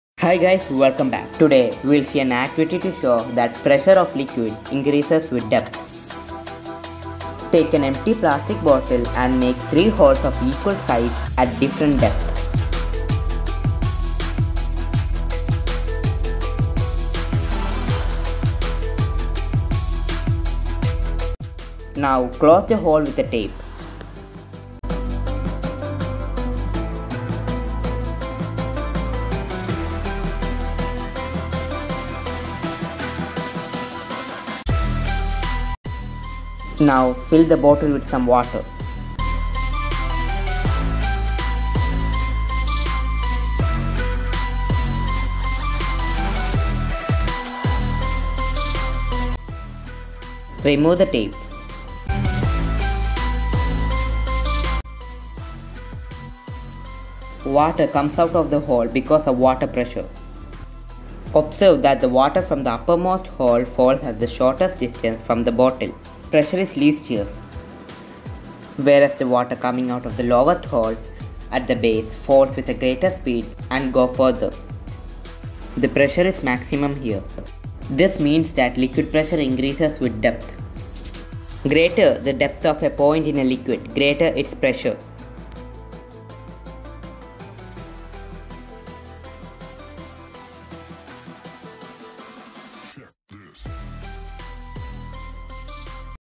Pressure of liquid increases with depth
Pressure increases with depth
An Introduction:
 The pressure exerted by a liquid increase with depth
 A liquid exerts the same pressure in all directions at a given depth
The experiment demonstrates how water pressure increases with depth.
1. Take a water container
2. Punch holes in side of the container at a gap of one inch.
3. Fill the container with water
4. Observe the distance from the container that the water jet comes out of each hole.
Observation:
The water jet from the lowest hole comes out with the greatest force and falls at maximum distance from the container.
Whereas the water from the top most hole will fall at the minimum distance from the container.

Conclusion:
The experiment concludes that water or any liquid exerts pressure on the side of the container and this pressure increases with increase in depth.
 Pressure and depth are directly proportional.